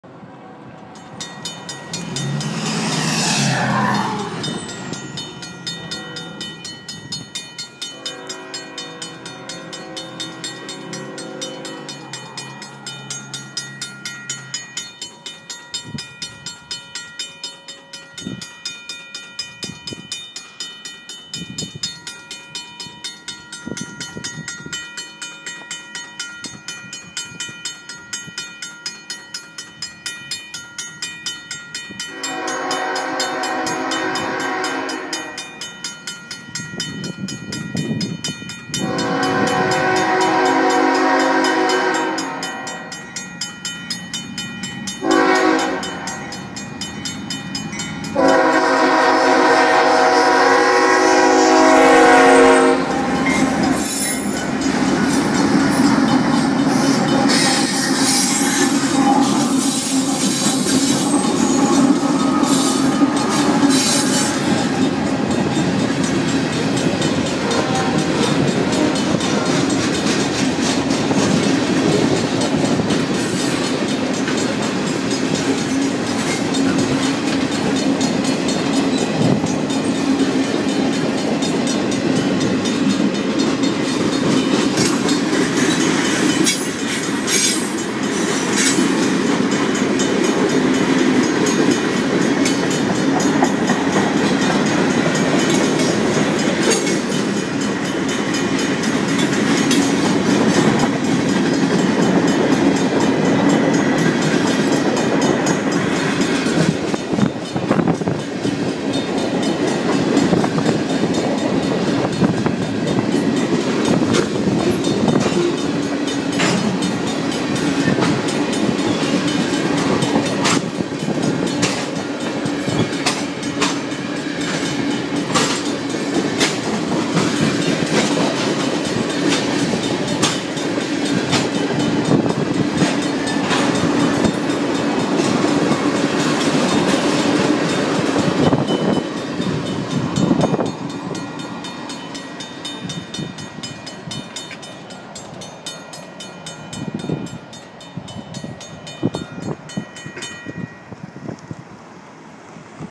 Whittier Street railroad crossing, UP 5408 Manifest, Whiting, IA
A short Union Pacific mixed freight heads north through Whiting. I chased them from Onawa.
Bells ring through the whole activation
Left signal has 1.5 pairs of WCH 12x24 inch LED lights, 1 WCH 12x20 inch LED light, a WCH gate mechanism, and Alstom LED gate lights
Right Signal has 2 pairs of WCH 12x24 inch LED lights, a WCH gate mechanism, Alstom LED gate lights, and a Safetran mechanical bell